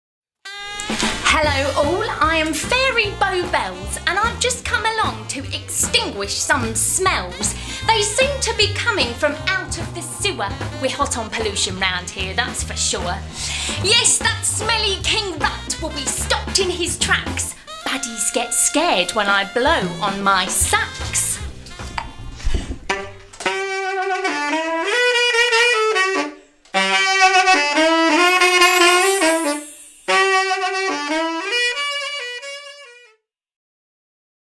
Fairy Bow Bells to the rescue! - Dick Whittington - The Central Theatre, Chatham
Not only has Fairy Bow Bells got some fantastic rhymes ... she plays a mean sax too!

Dick Whittington - 7-31 December 2018 -  The Central Theatre, Chatham